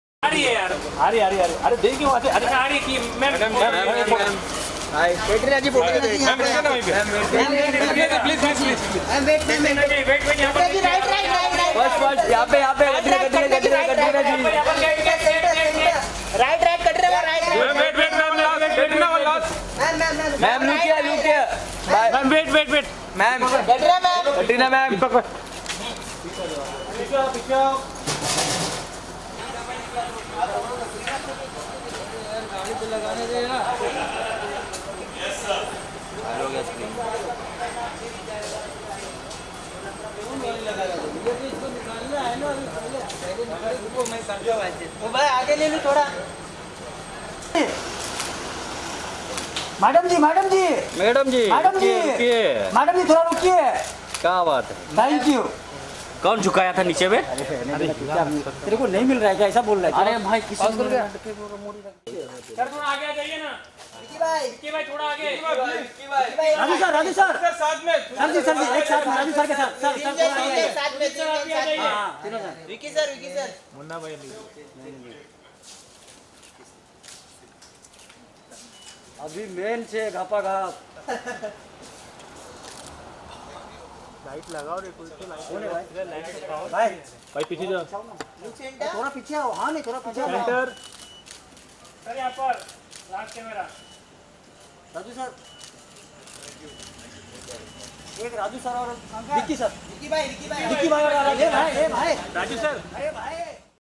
Katrina Kaif पहुंची Boyfriend VICKY KAUSAL की Film Udham Singh देखने
Katrina Kaif Arrived at Sardar Udham Singh Screening in Juhu today.